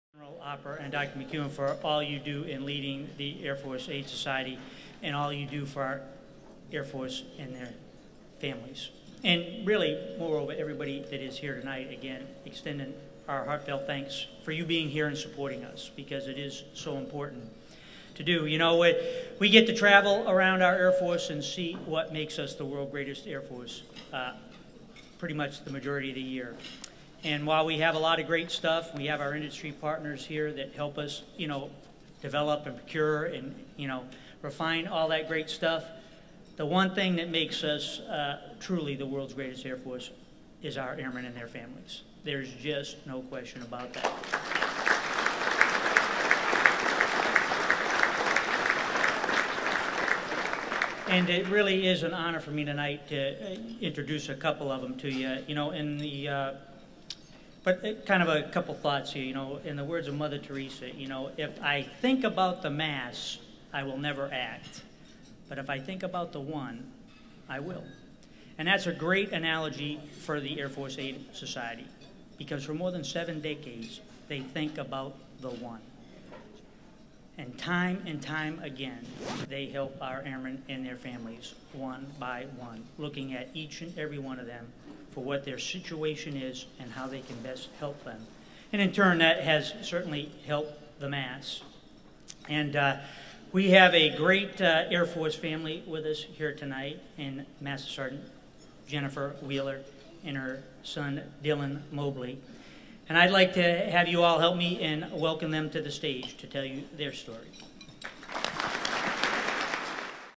CMSAF James A. Cody
Speaking at the 2014 Air Force Charity Ball